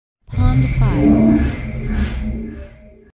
Futuristic Train - SOUND EFFECT
This is a sound that resembles a steam train of the future, it could serve as a topper for any project.